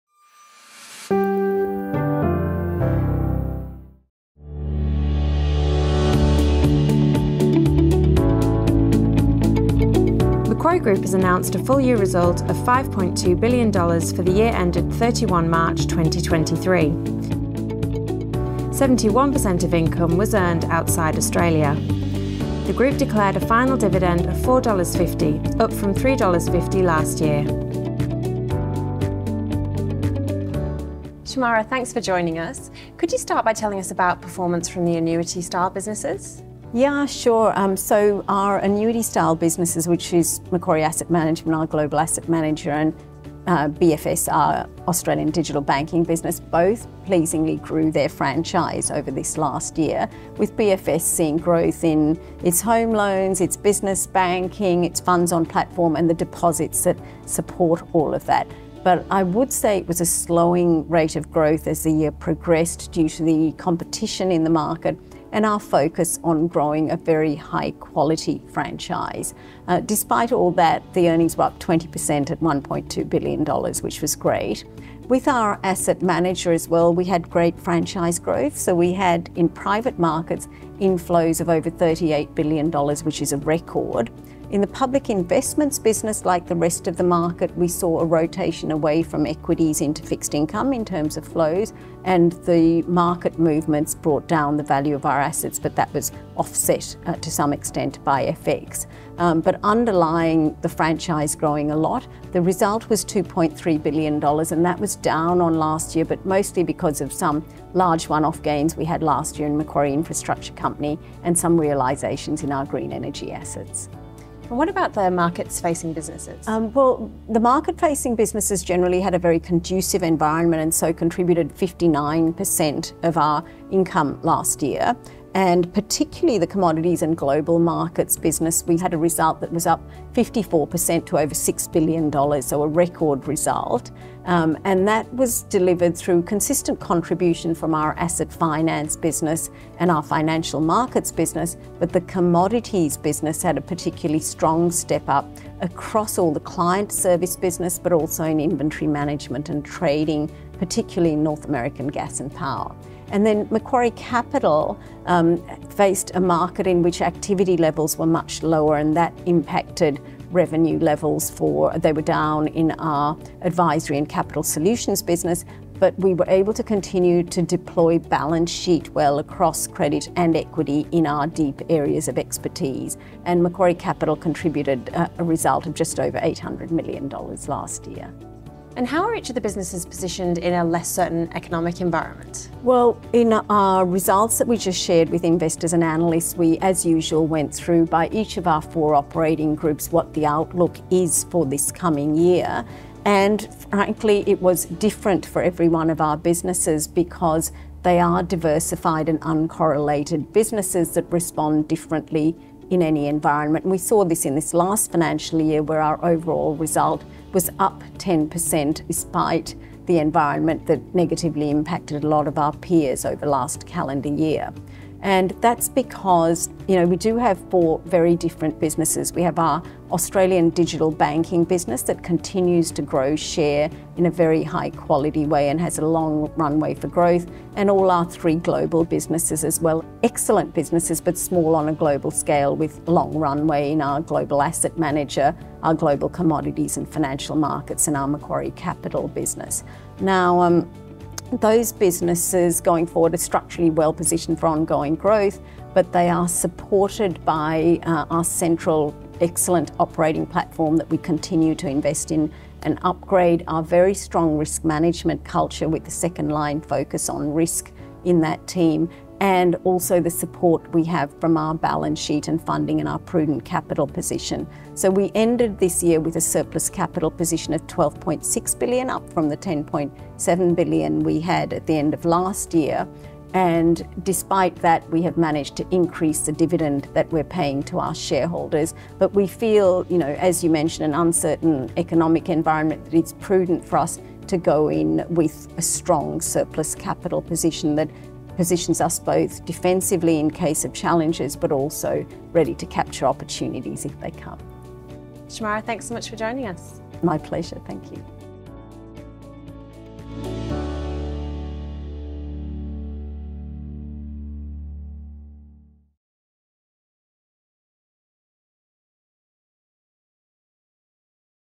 2023 Full-Year Result CEO Overview | Macquarie Group
Shemara Wikramanayake, Macquarie Group Managing Director and CEO, discusses the Group’s 2023 full-year result.